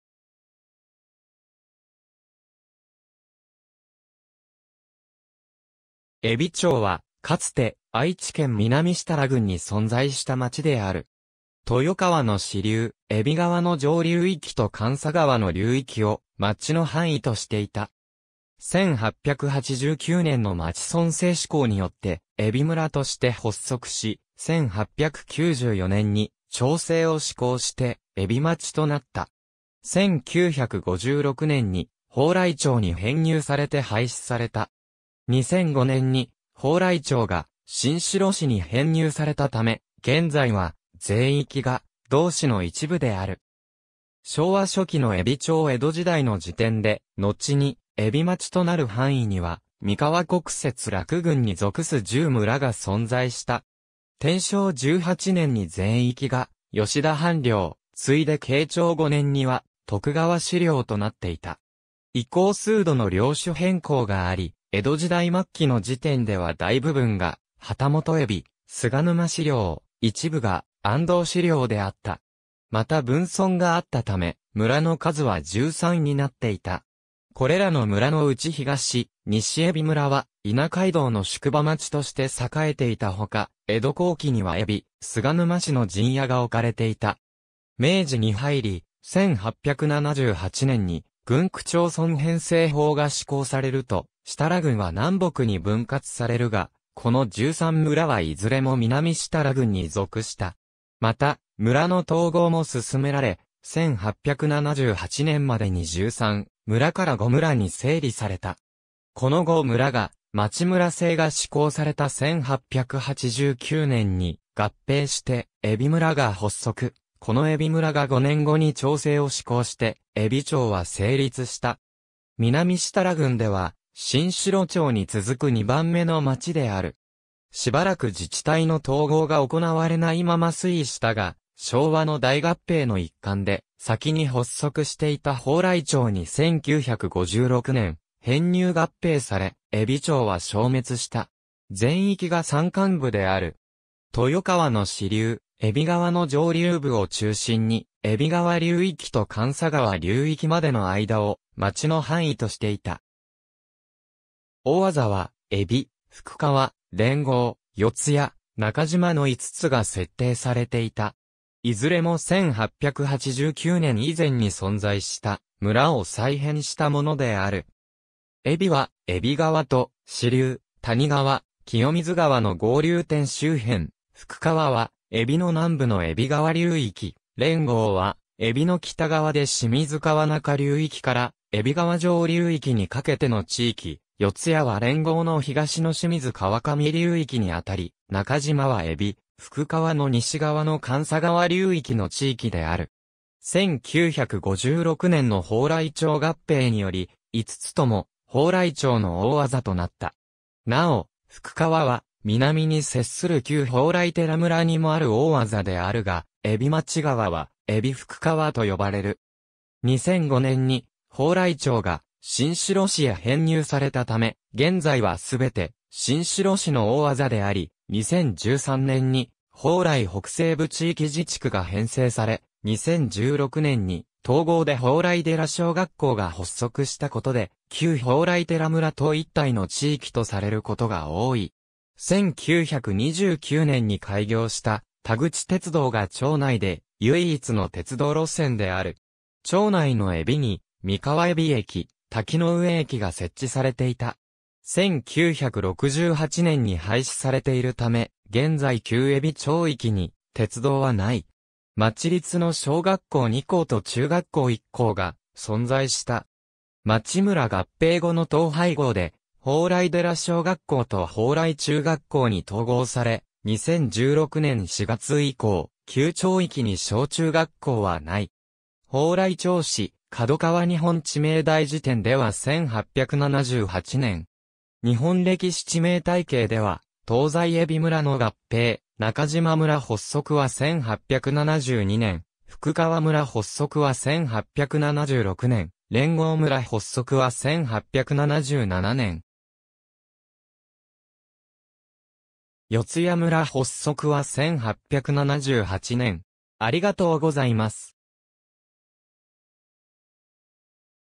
海老町
海老町（えびちょう）は、かつて愛知県南設楽郡に存在した町である。
豊川（寒狭川）の支流海老川の上流域と寒狭川の流域を町の範囲としていた。
1889年（明治22年）の町村制施行によって海老村として発足し、1894年（明治27年）に町制を施行して海老町となった。
1956年（昭和31年）に鳳来町に編入されて廃止された。
2005年（平成17年）に鳳来町が新城市に編入されたため、現在は全域が同市の一部である。
昭和初期の海老町 江戸時代の時点で、のちに海老町となる範囲には三河国設楽郡に属す10村が存在した。
天正18年（1590年）に全域が吉田藩領（池田輝政領）、ついで慶長5年（1600年）には徳川氏領（幕府成立後は幕府領）となっていた。
以降数度の領主変更があり、江戸時代末期の時点では大部分が旗本海老菅沼氏領、一部が安藤氏領（磐城平藩領）であった。
また分村があったため、村の数は13になっていた。
これらの村のうち東・西海老村は伊那街道（中馬街道）の宿場町として栄えていたほか、江戸後期には海老菅沼氏の陣屋が置かれていた。
明治に入り、1878年（明治11年）に郡区町村編制法が施行されると設楽郡は南北に分割されるが、この13村はいずれも南設楽郡に属した。
また、村の統合も進められ、1878年（明治11年）までに13村から5村に整理された。
この5村が、町村制が施行された1889年（明治22年）に合併して海老村が発足、この海老村が5年後に町制を施行して海老町は成立した。
南設楽郡では新城町に続く2番目の町である。
しばらく自治体の統合が行われないまま推移したが、昭和の大合併の一環で、先に発足していた鳳来町に1956年（昭和31年）編入合併され、海老町は消滅した。
全域が山間部である。
豊川（寒狭川）の支流海老川の上流部を中心に、海老川流域と寒狭川流域までの間を町の範囲としていた。
大字は「海老」「副川」「連合」「四谷」「中島」の5つが設定されていた。
いずれも1889年以前に存在した村を再編したものである。
「海老」は海老川と支流谷川・清水川の合流点周辺、「副川」は海老の南部の海老川流域、「連合」は海老の北側で清水川中流域から海老川上流域にかけての地域、「四谷」は連合の東の清水川上流域にあたり、「中島」は海老・副川の西側の寒狭川流域の地域である。
1956年の鳳来町合併により、5つとも鳳来町の大字となった。
なお、「副川」は南に接する旧鳳来寺村にもある大字であるが、海老町側は「海老副川」と呼ばれる。
2005年に鳳来町が新城市へ編入されたため、現在はすべて新城市の大字であり、2013年に鳳来北西部地域自治区が編成され、2016年に統合で鳳来寺小学校が発足したことで、旧鳳来寺村と一体の地域とされることが多い。
1929年（昭和4年）に開業した田口鉄道（後の豊橋鉄道田口線）が町内で唯一の鉄道路線である。
町内の海老に、三河海老駅・滝上駅が設置されていた。
1968年（昭和43年）に廃止されているため、現在旧海老町域に鉄道はない。
町立の小学校2校と中学校1校が存在した。
町村合併後の統廃合で、鳳来寺小学校と鳳来中学校に統合され、2016年4月以降、旧町域に小中学校はない。
^ 『鳳来町誌』『角川日本地名大事典』では1878年。
『日本歴史知名体系』では、東西海老村の合併・中島村発足は1872年（明治5年）、副川村発足は1876年（明治9年）、連合村発足は1877年（明治10年）。
四谷村発足は1878年。